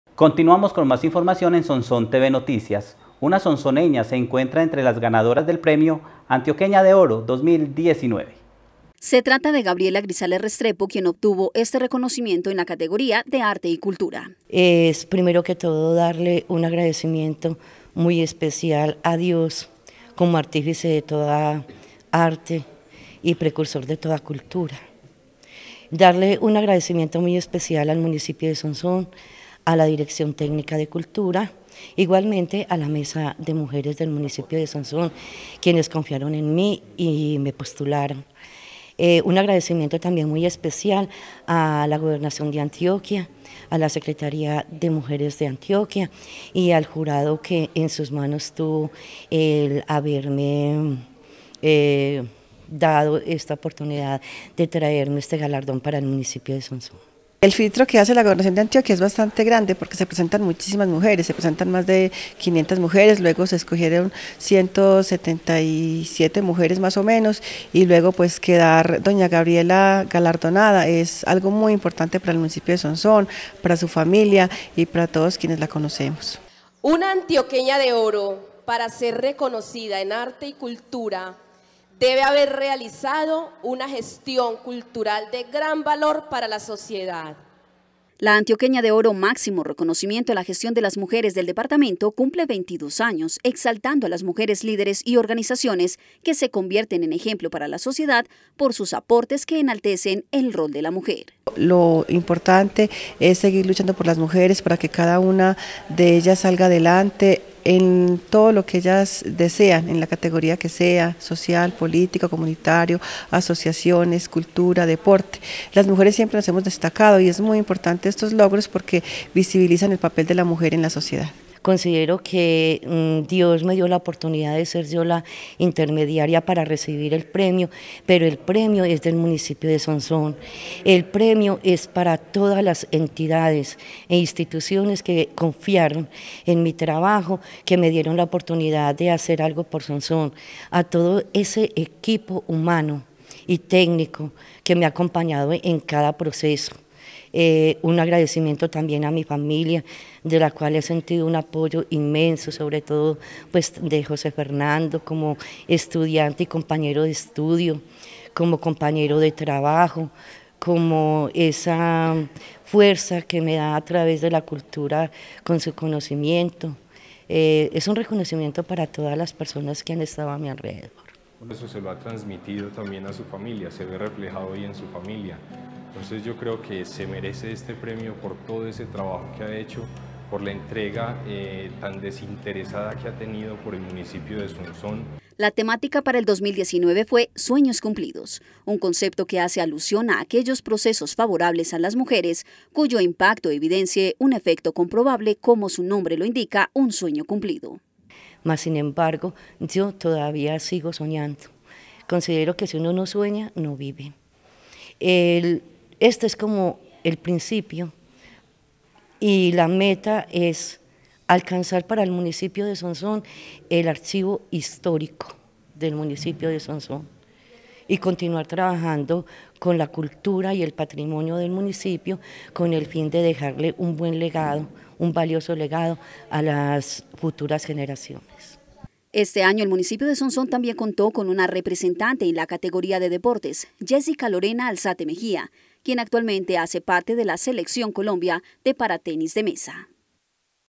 GABRIELA GRISALES RESTREPO, ANTIOQUEÑA DE ORO 2019
Una sonsoneña se encuentra entre las ganadoras del premio Antioqueña de Oro 2019.